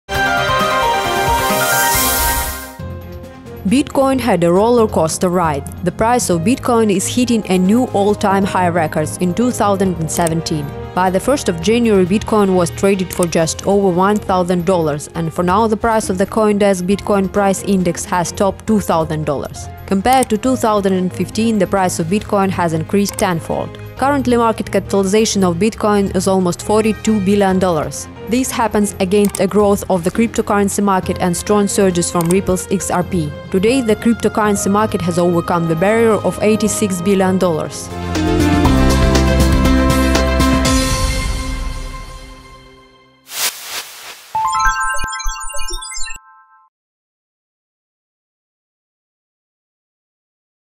KCN Bitcoin is doubling its price in 2017
Bitcoin had a roller coaster ride. The price of bitcoin is hitting a new all-time high records in 2017. By the first of January bitcoin was traded for just over one thousand dollars and for now the price on the CoinDesk Bitcoin Price Index (BPI) has topped two thousand dollars. Compared to 2015 the price of bitcoin has increased tenfold. Currently market capitalization of bitcoin is almost forty two billion dollars. This happens against a growth of the cryptocurrency market and strong surges from Ripple's XRP. Today the cryptocurrency market has overcome the barrier of eighty six billion dollars.